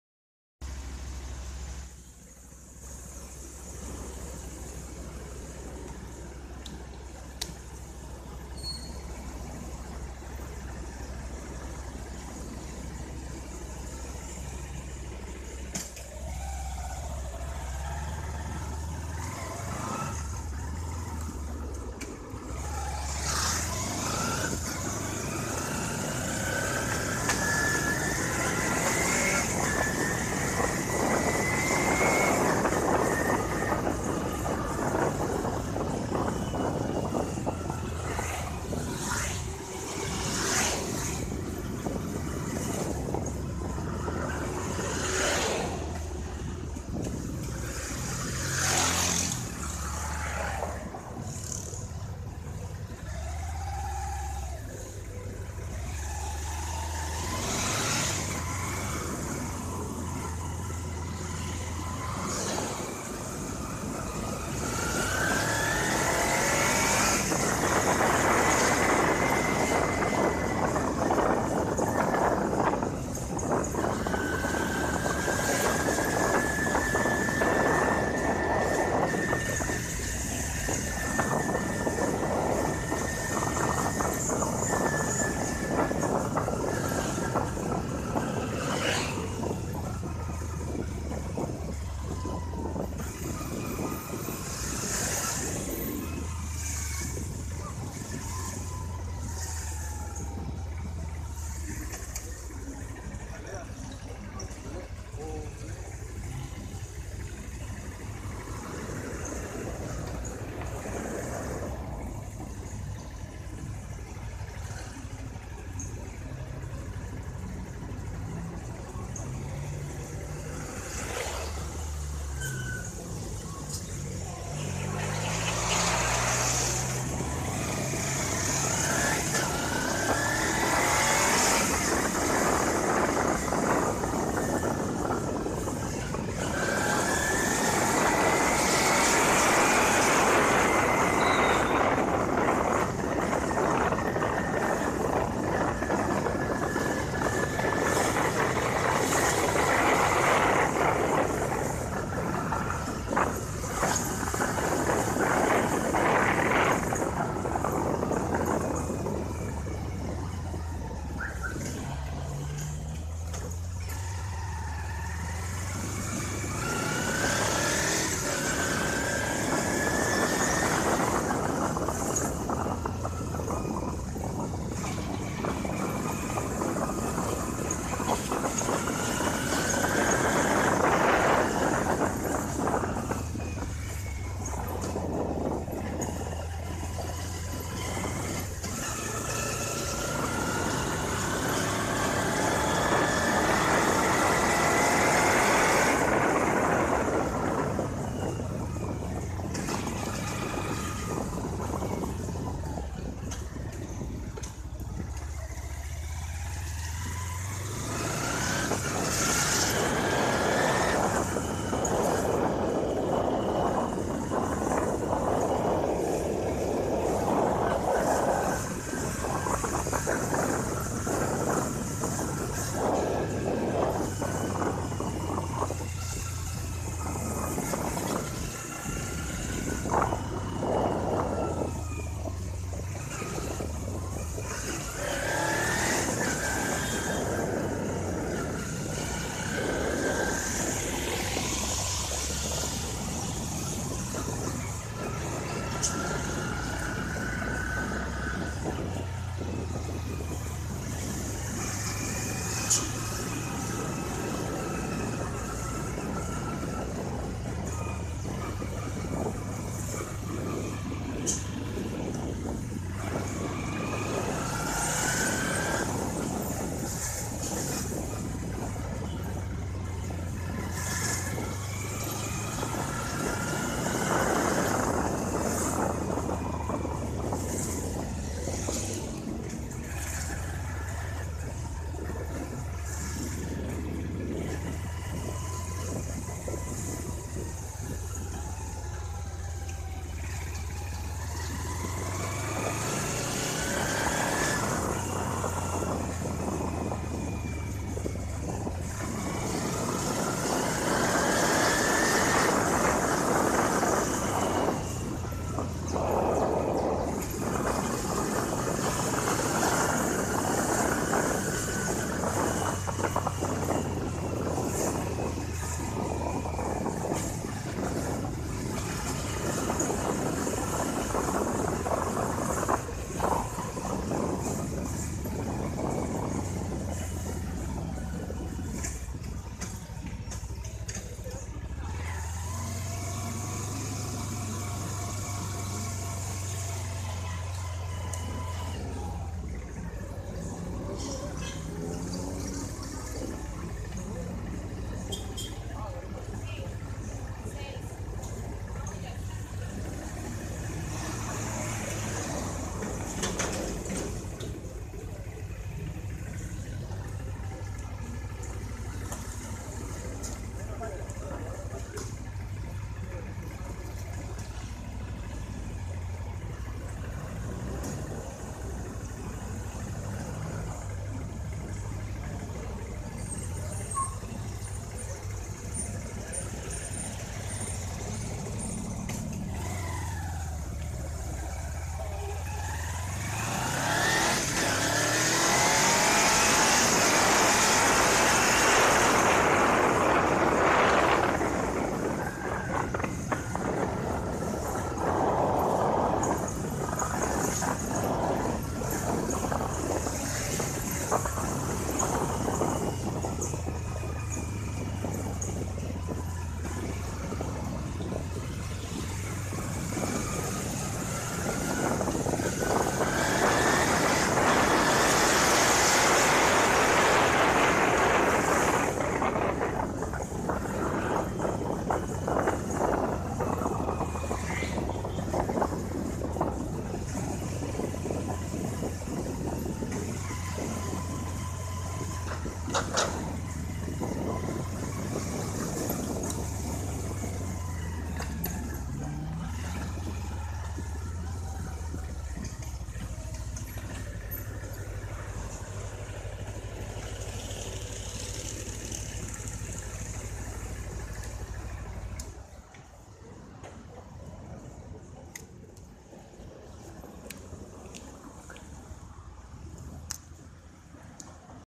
Prueba piloto para grabación de viajes: recorrido por la vecindad donde vivo.
Recorrido piloto para probar grabación para futuros viajes: recorrido por San Fernando Cuba, Gama y la Villa Olímpica de Pereira.